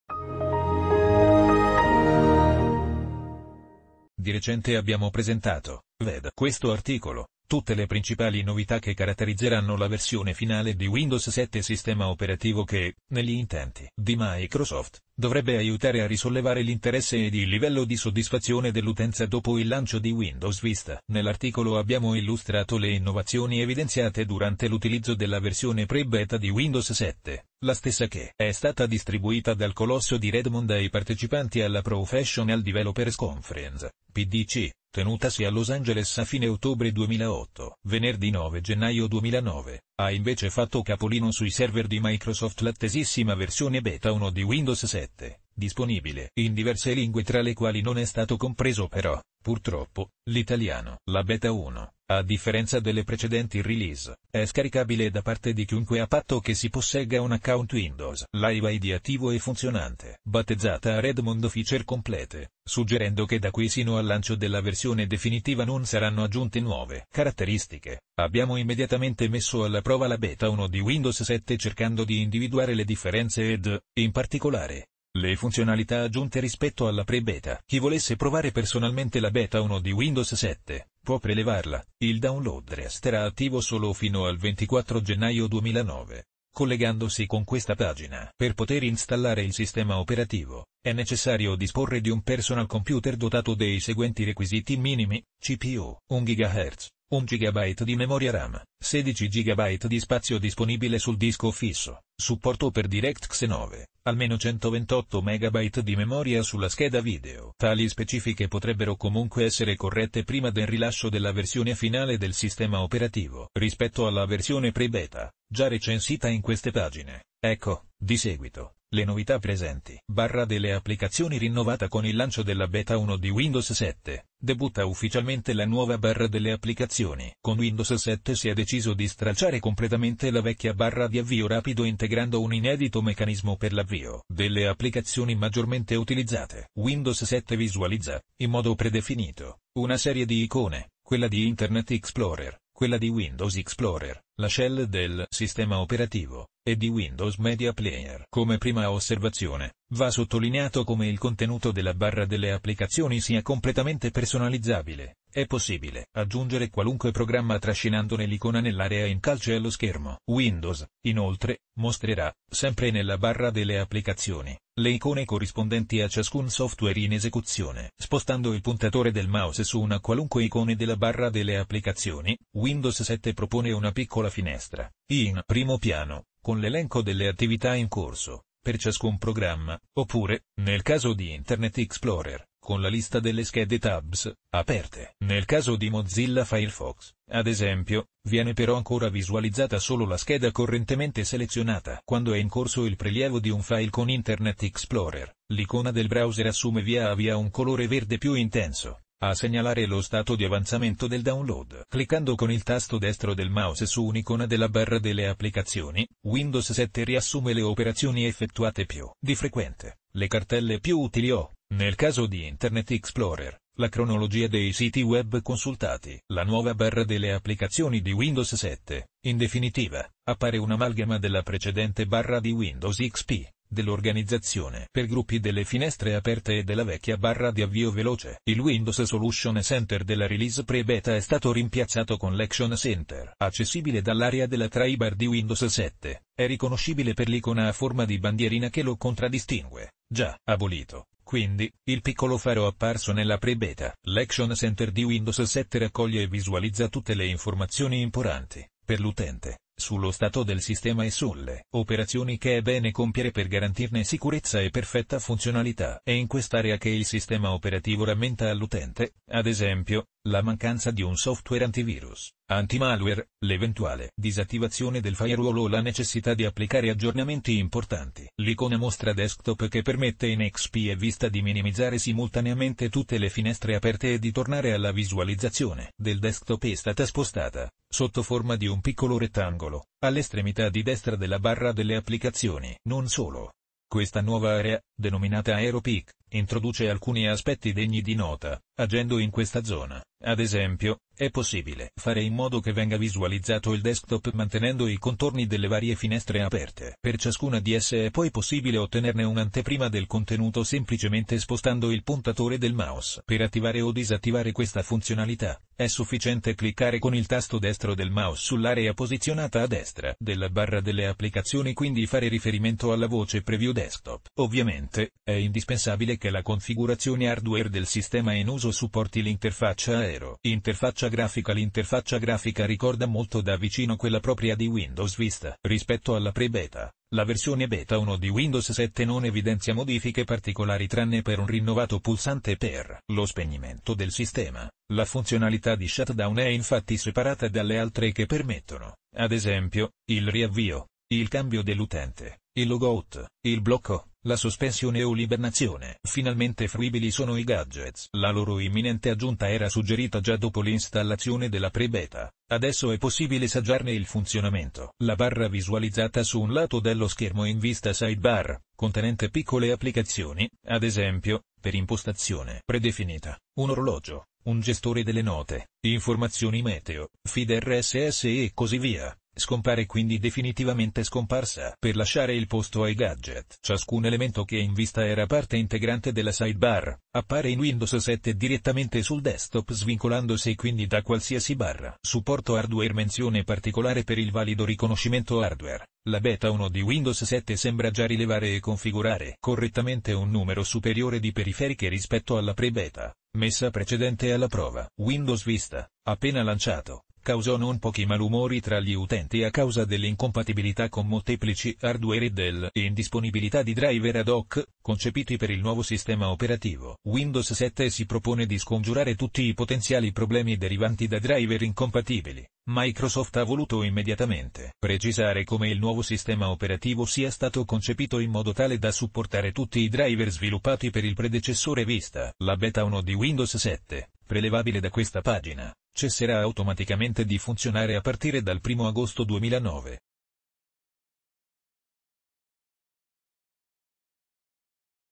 Windows 7: le novità della nuova versione di anteprima
Scopri tutte le ultime novità sulla nuova versione di anteprima di Windows 7! In questo video, esploriamo le caratteristiche aggiornate, le migliorie di sicurezza e le nuove funzionalità che rendono Windows 7 un sistema operativo moderno e affidabile. Se sei un appassionato di tecnologia o semplicemente curioso di sapere cosa offre la prossima evoluzione di Windows, non perdere questa anteprima esclusiva!